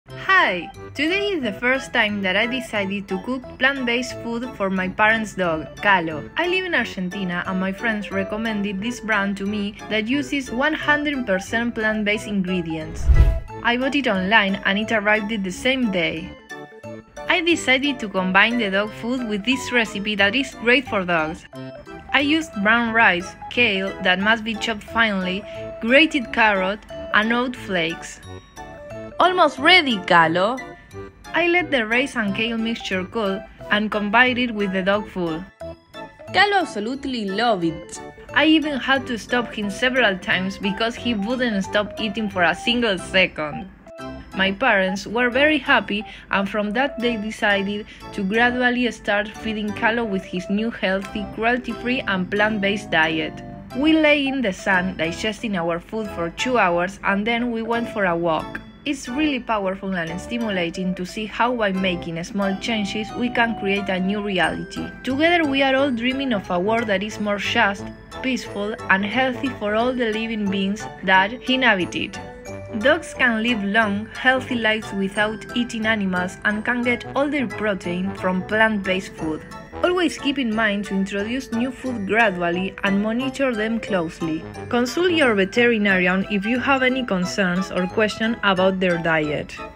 Kalo Tried VEGAN DOG Food For The FIRST TIME
Did you know dogs can not only survive but thrive well on a plant-based diet?

Here’s the story of Kalo the dog who tried vegan dog food with for the first time and can’t get enough of it.